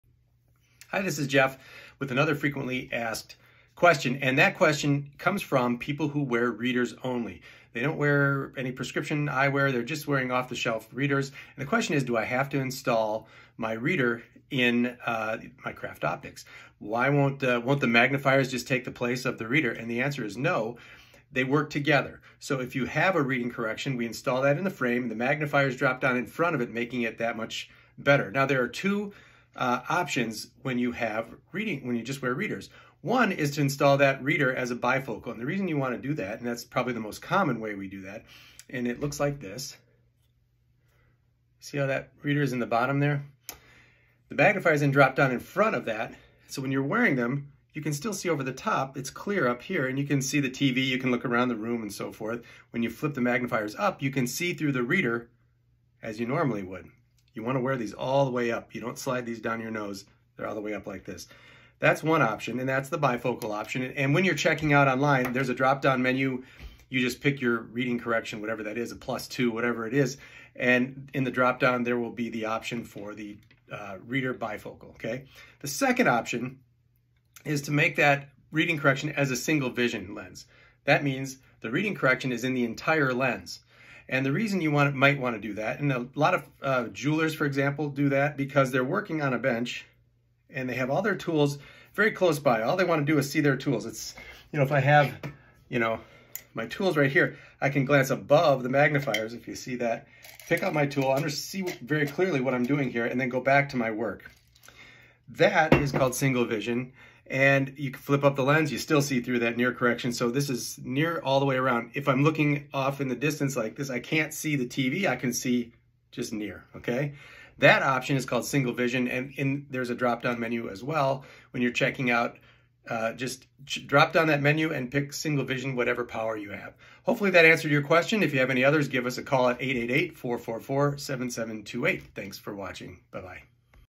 FAQ: I only wear readers/cheaters. What are the lens choices for my CraftOptics?
If you wear readers only, you definitely want to have that reading correction installed in your CraftOptics frames. We always remind people that CraftOptics is an ENHANCEMENT to your correction (reader or prescription), not a substitute for it.

Remember, CraftOptics magnifiers flip up and down. When you flip them up, you want to see through your reading correction as you normally do. When you drop them in the down position, they work WITH your reading correction--they actually drop down right in front of it, making your near vision so much better.

In this video, Jeff explains the two main choices people make for readers installed in their CraftOptics frames:

1. As a bifocal: Your reader is installed at the bottom of the frame and it will be clear above. This allows you to see the TV or around your room when glancing over the top of the magnifiers, for example.

2. As a single vision near lens: This option has your reader installed in the entire frame, so when you glance over the magnifiers through the upper part of your frame, you will still see through that reader. This allows you to see your tools or anything else you need to see that is nearby, within 14-16" or so.

Each option is presented during checkout in a dropdown menu.